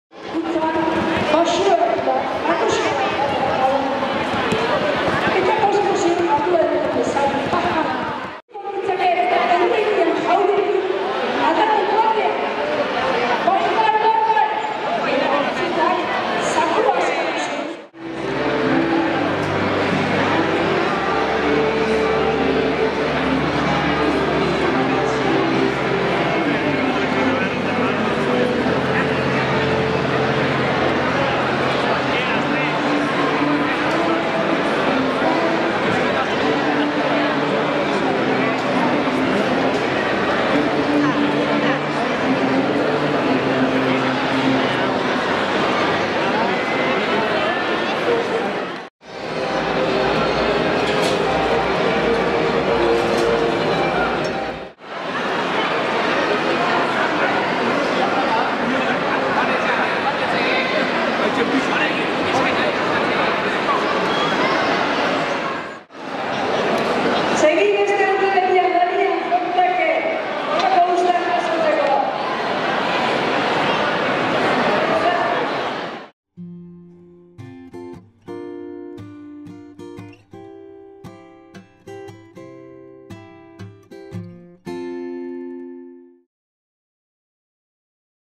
Santomas Azoka 2017
Abenduaren 17an ospatu zen Santomas Azoka Usurbilen.